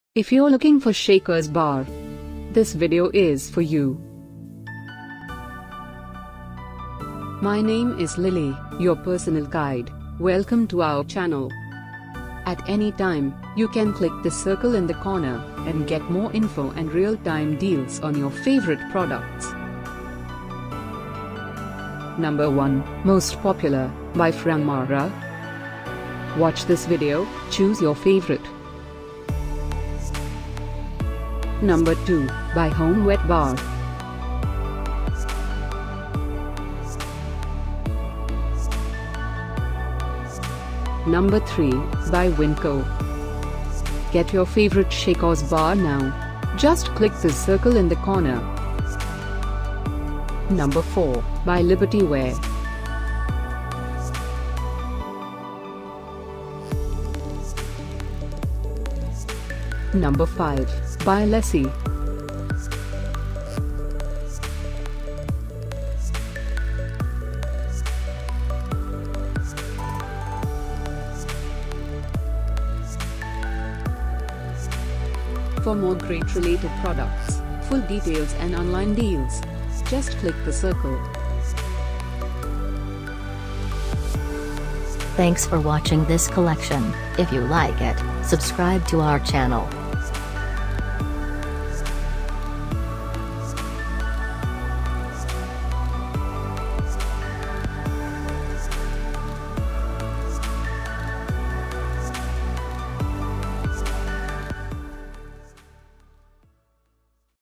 Top 10 Shakers Bar [2018]: Recipe Cocktail Shaker, Stainless Steel/Glass
Top 10 Shakers Bar [2018]

Recipe Cocktail Shaker, Stainless Steel/Glass by Franmara

Professional Boston Shaker Home Bar Set by HomeWetBar

KegWorks Boston Cocktail Bar Shaker, Stainless Steel 30-Ounce by Winco

Libertyware Stainless Steel Milkshake Collar (04-0478) Category: Bar Shakers by Libertyware

Alessi Chiringuito Cocktail Shaker by Alessi

Roman Olive Oil & Vinegar Clear Glass Bottle Tabletop Kitchen Set - G57 Set of 2, Clear by Knobs More Home Decor

5-pc Cocktail Shaker Gift Set ~ Clear Glass 20-ounce Mason Cocktail Shaker Stainless Steel by GF Art